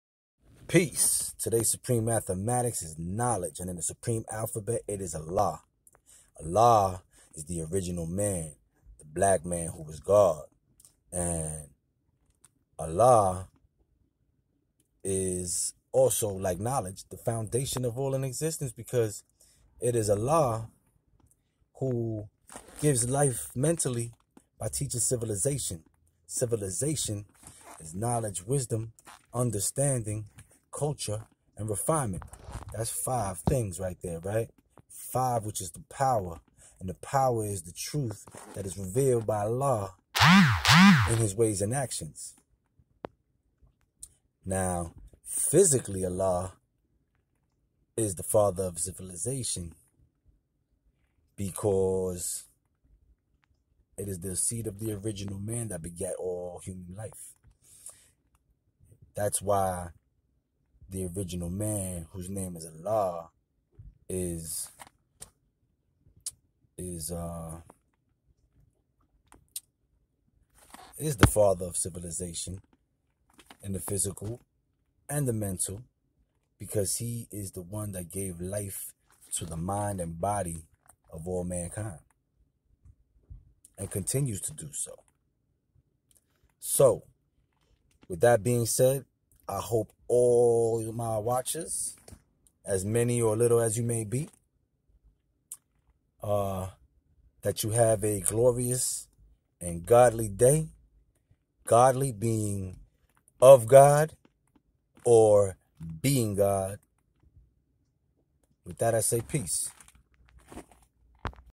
Supreme Alphabet. - Today's degree (A) = Allah, The Mental & Physical Father Of Civilization.
Peace. I come in the divine name of True Self Peace Allah.
We are now exploring  the Supreme Alphabet which is a continuance of the Supreme Mathematics and the and the principles that govern the reality of the universe. Together they are the language of Allah's 5%.
Today's Supreme Mathematics is knowledge (1) and the knowledge degree in the Supreme Alphabet is (A) = Allah. Allah is the name of the Original Man from which all human families originated from so He is knowledge which is the foundation.
P.E.A.C.E.